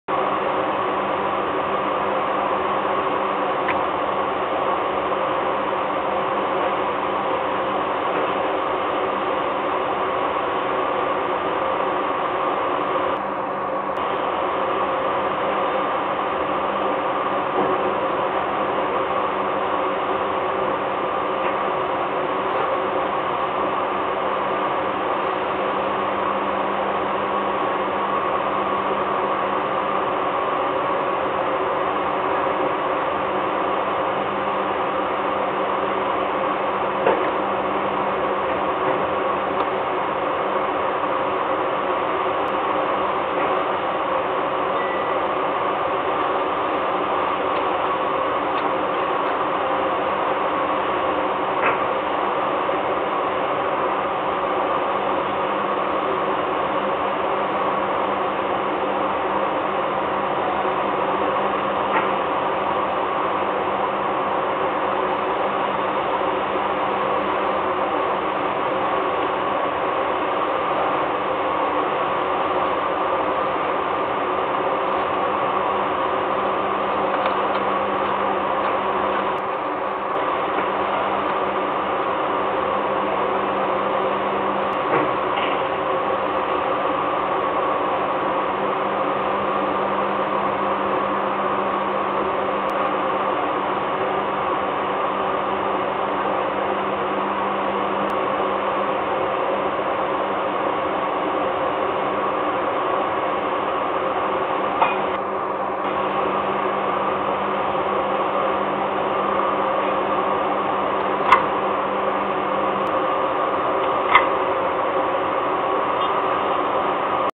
szybka kontrola Gdynia EDF Gniazdo 2016
Bryza w siedem minut sprawdziła żwir w gnieździe ...